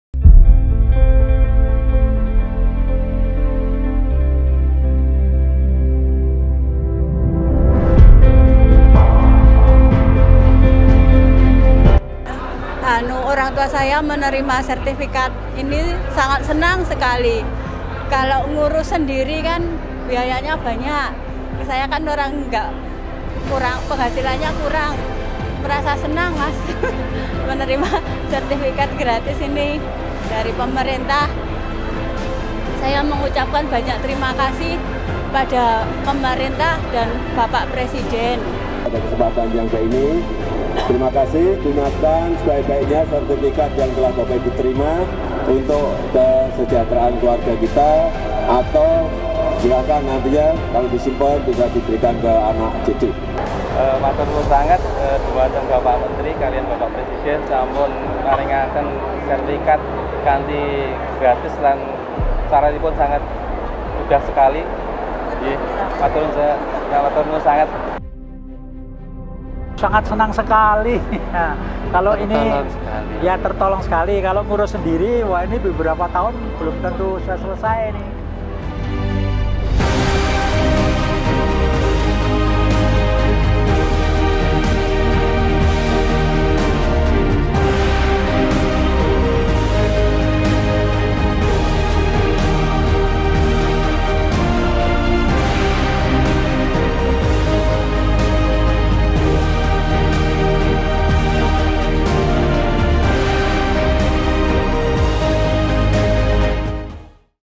PRESIDEN RE SERAHKAN SERTIPIKAT TANAH DI YOGYAKARTA
bpn.go.id